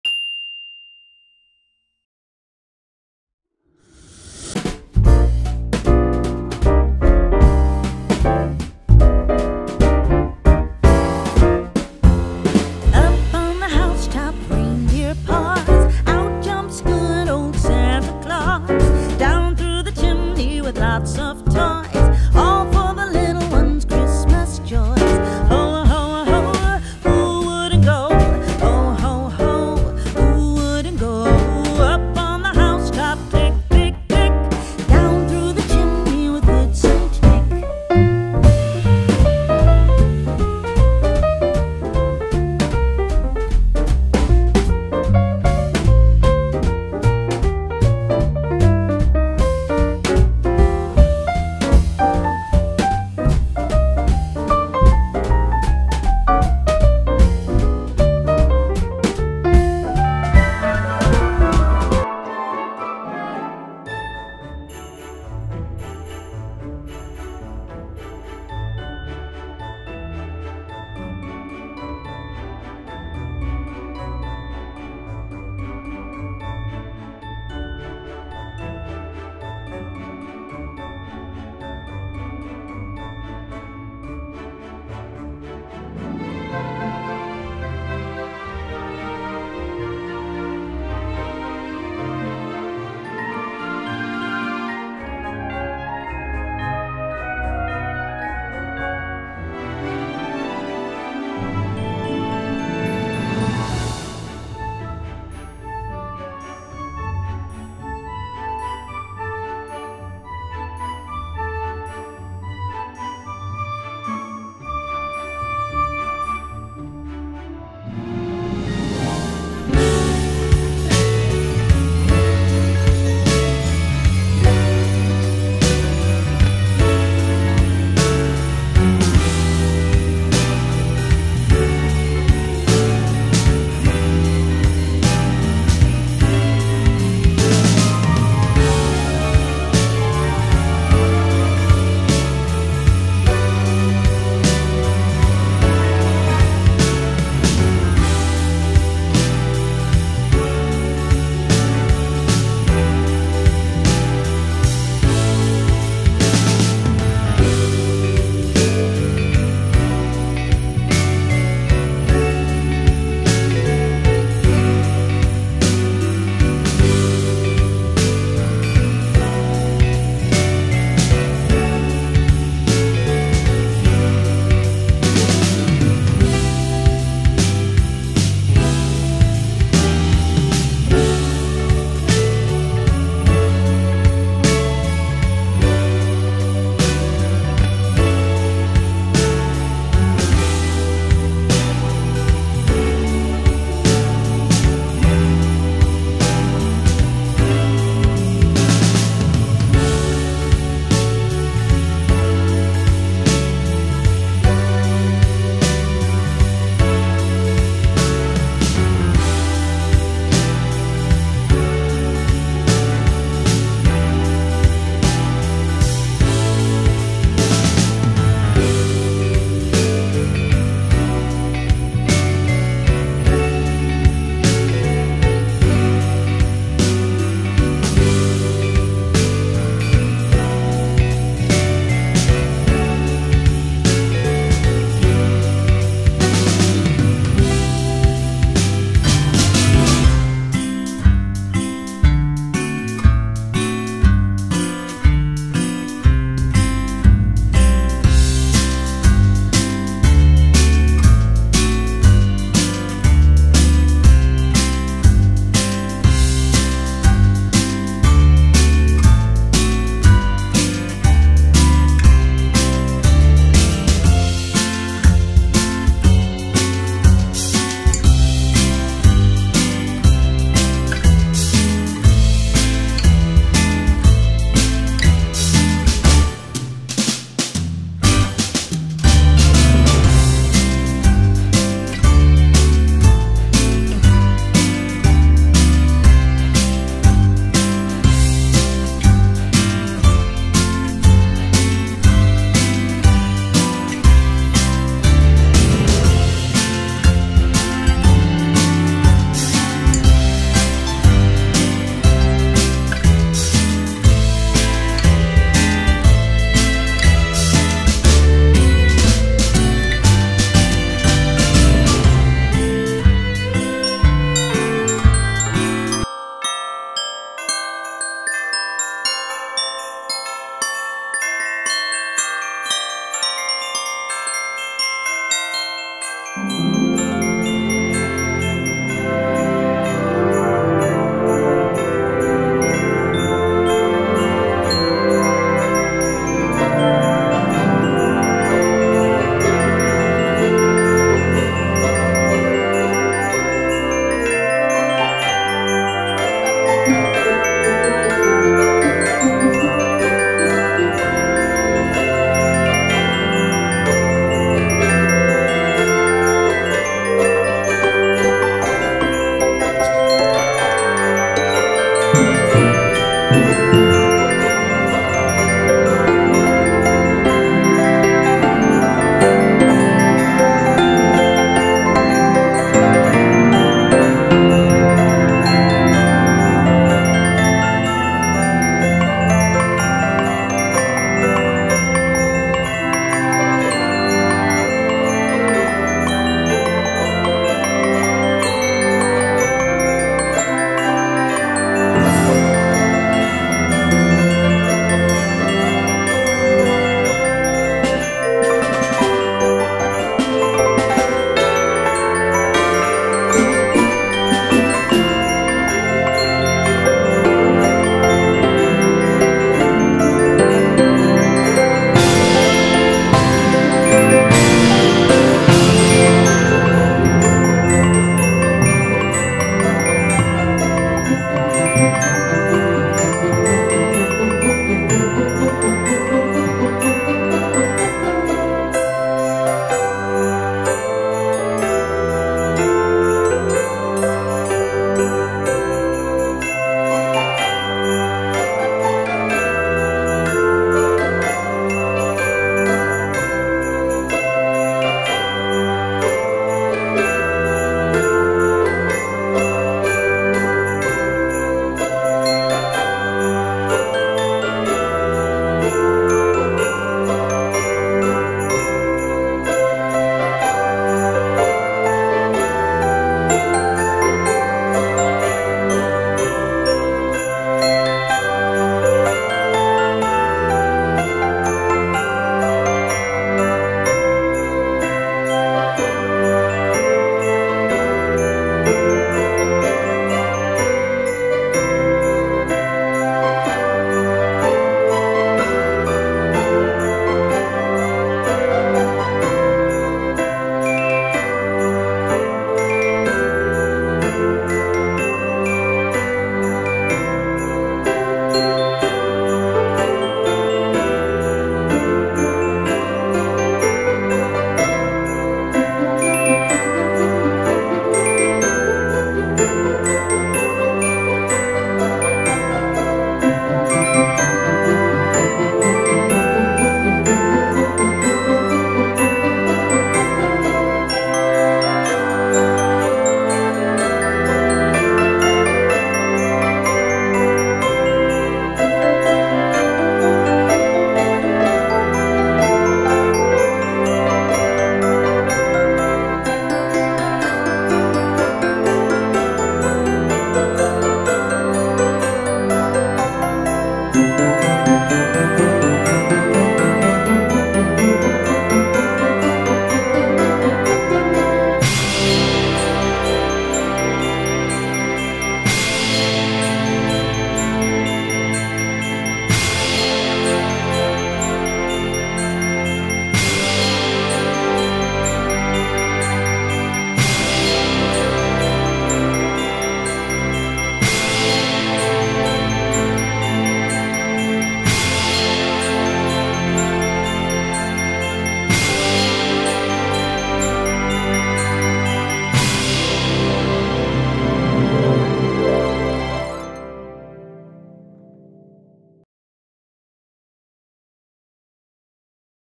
20 Celebrities Christmas photos with their families.
This video is celebrities Christmas photos and videos with their love once